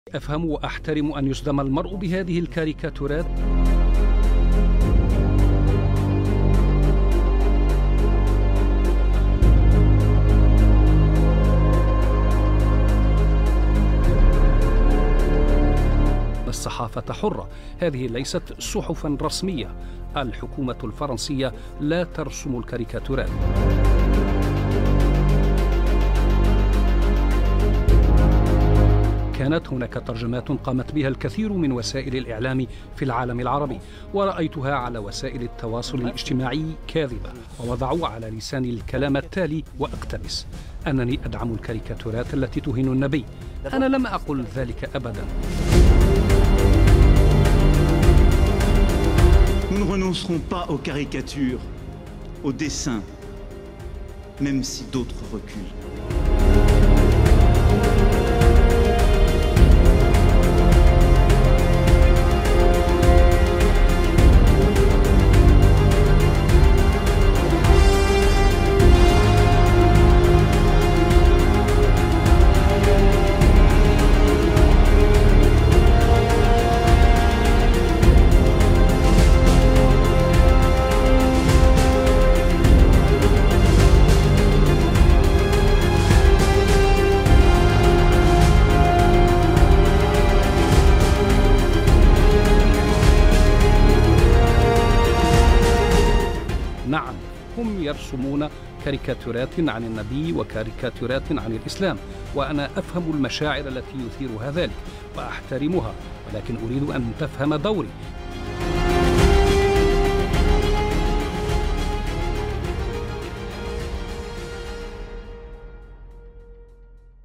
"لن تخدعنا ماكرون"
ماكرون يتراجع ويدعي أن تصريحاته جرى تحريفها ومغردون